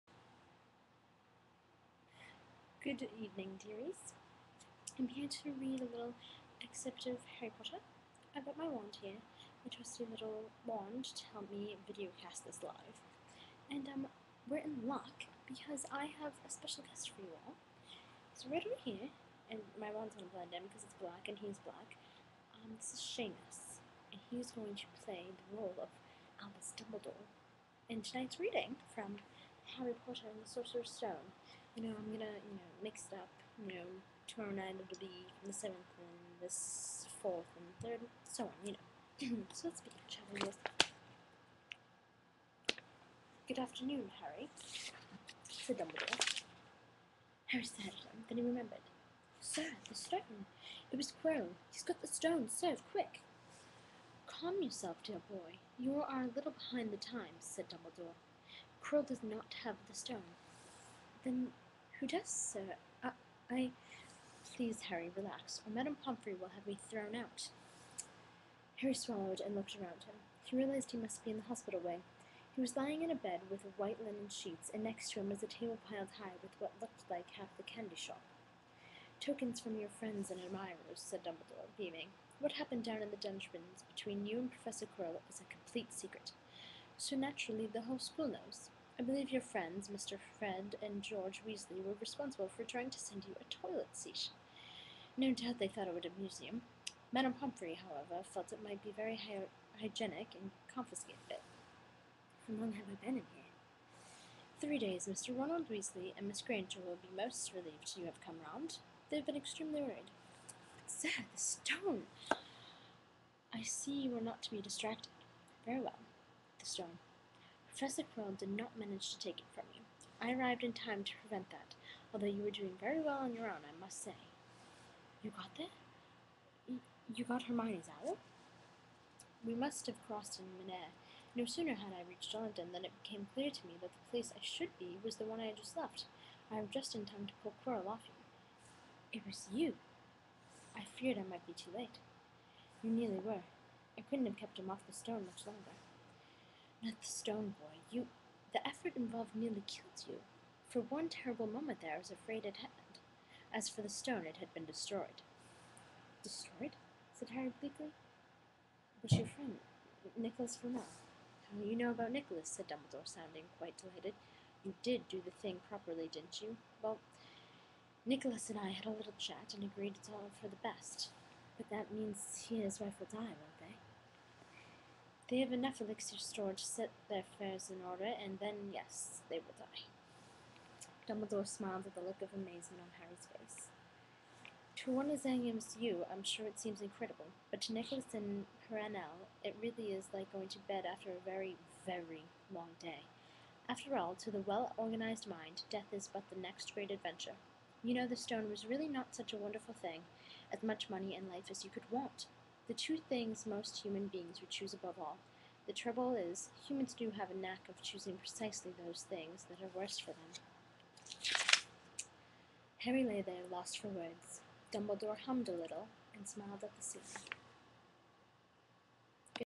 Harry Potter and the Sorcerer's Stone Reading #1
My first HP reading for Youtube! Enjoy.

Featuring: Seamus O'Tool Cote